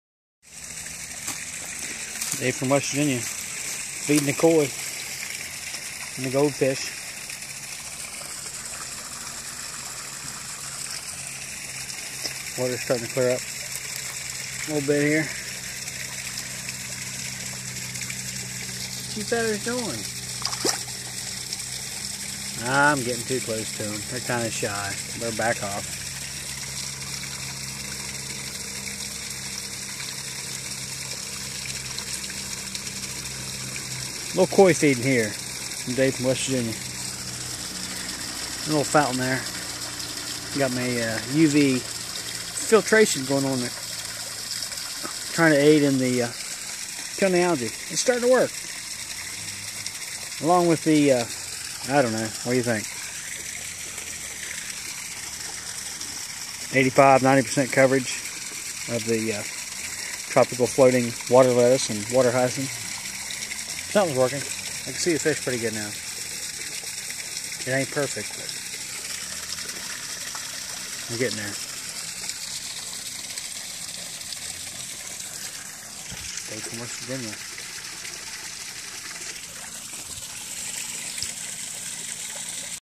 Koi Pond 10-6-15
Dave from WV feeding his fish